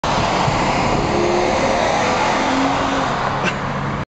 BMW 535i Gran Turismo
Getting passed by a new 2011 535i gran turismo